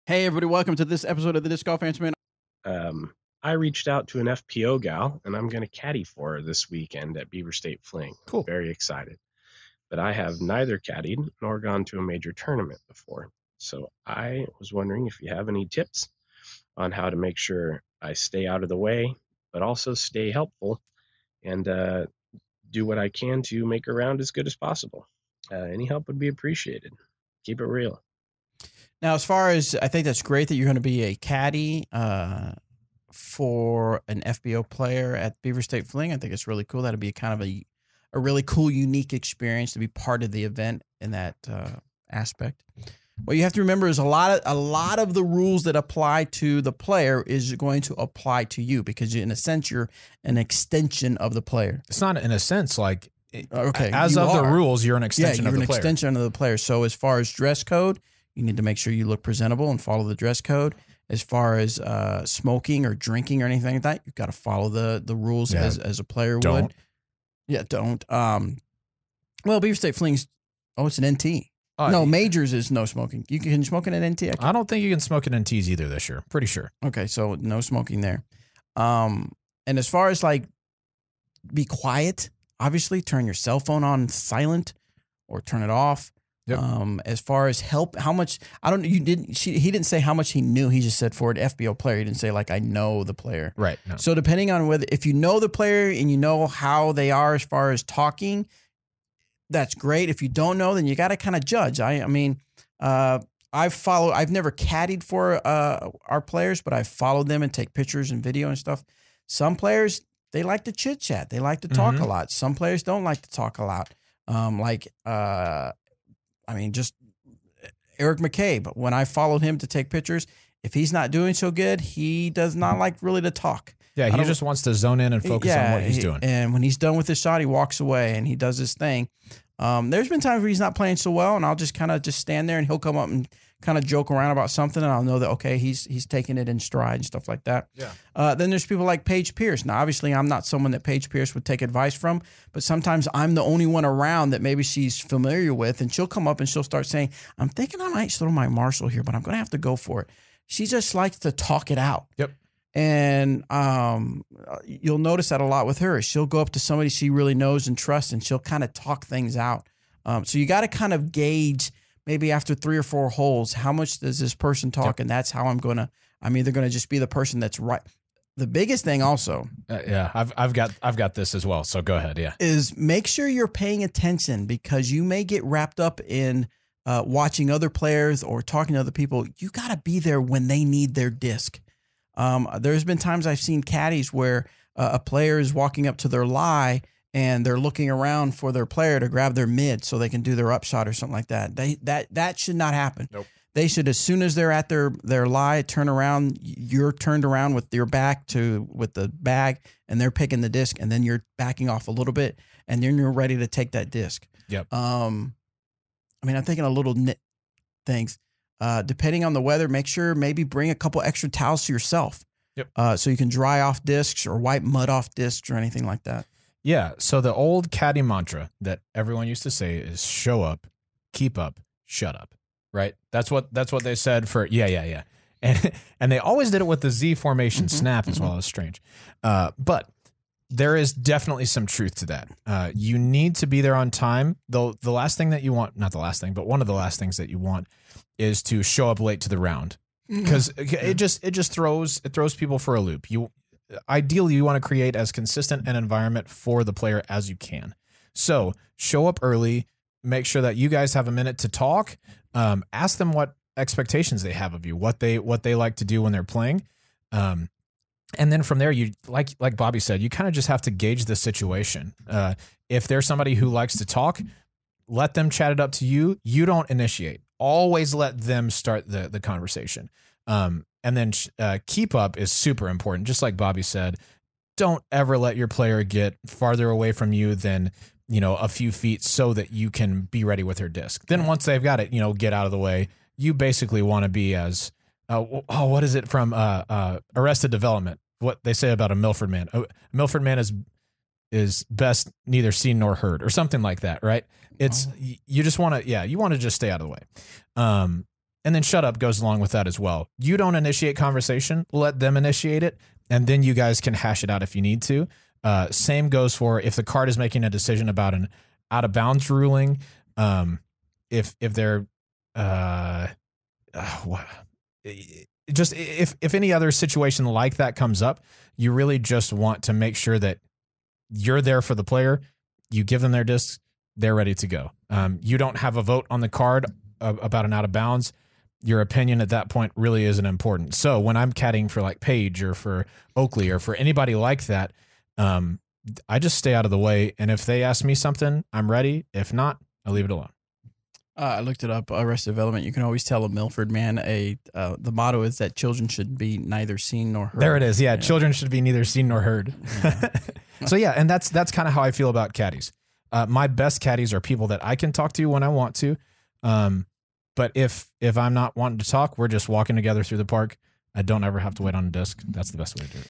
What do I need to know for my first time Caddying?
Robert and Bobby answer your questions and give some disc golf tips! Send in your questions at DISCGOLFANSWERMAN.COM!